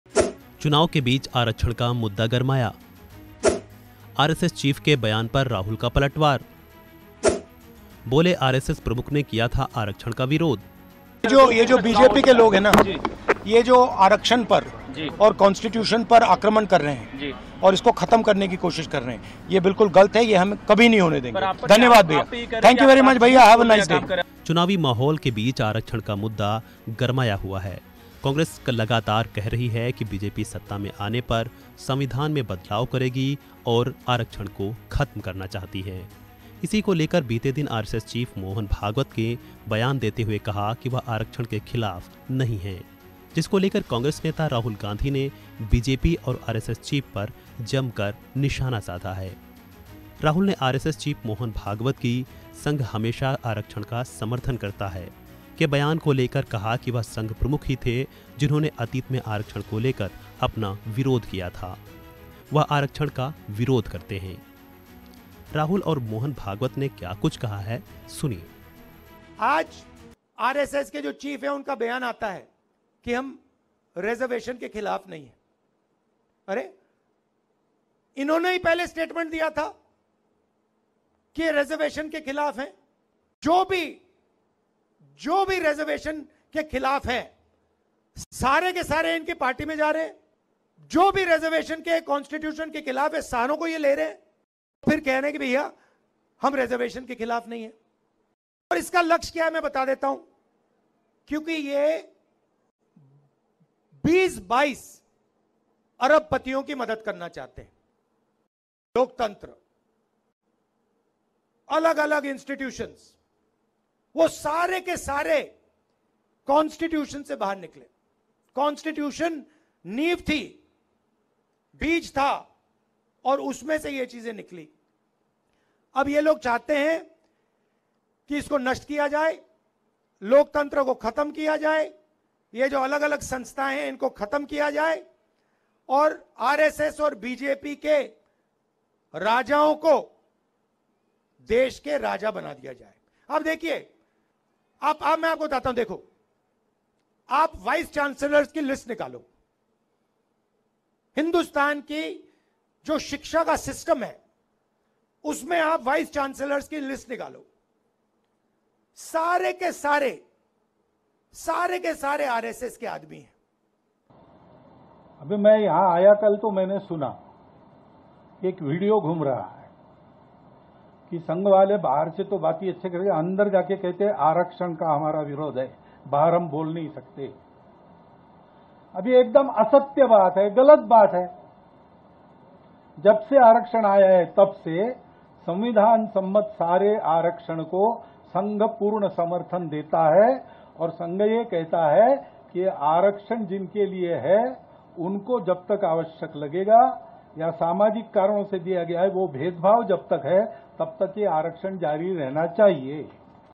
RSS Chief Mohan Bhagwat के Reservation बयान पर Rahul Gandhi का पलटवार | Amit Shah | वनइंडिया हिंदी
Lok Sabha Elections 2024: राहुल गांधी ने राष्ट्रीय स्वयंसेवक संघ (RSS) पर निशाना साधा है. राहुल गांधी (Rahul Gandhi) ने आरएसएस चीफ मोहन भागवत (RSS Chief Mohan Bhagwat) की ‘‘संघ हमेशा आरक्षण का समर्थन (Mohan Bhagwat ON Reservation) करता है’’ बयान को लेकर कहा कि वह संघ प्रमुख ही थे, जिन्होंने अतीत में आरक्षण को लेकर अपना विरोध व्यक्त किया था